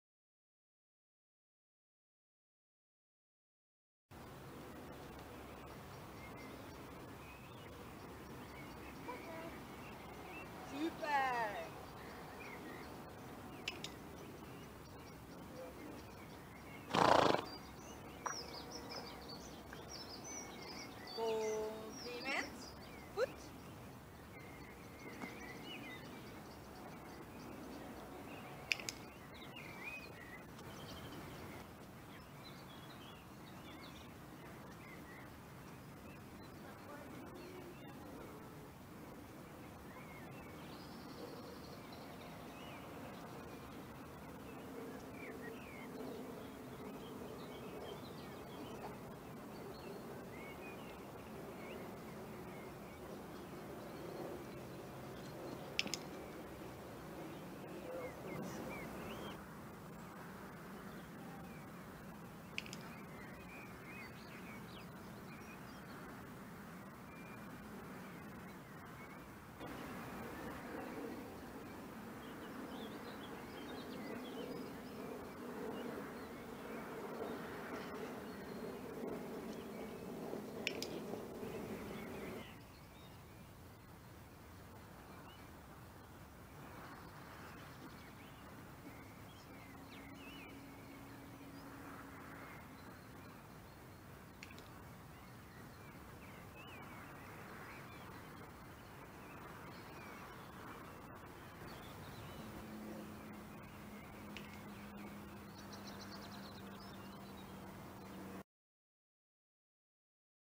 Horse agility & vrijheidsdressuur training met Swa
Een paar beelden van onze training van 15 mei 2017. Swa had er zin in en sprong zelfs een keer over een pion heen toen ik hem er omheen wilde sturen haha! (Staat niet goed op beeld helaas.) In dit filmpje oefenen we met een onderdeel van onze scary corner (balonnen aan een touwtje), het springen, wippen, spagaat en compliment en aan het einde zie je de heuptarget.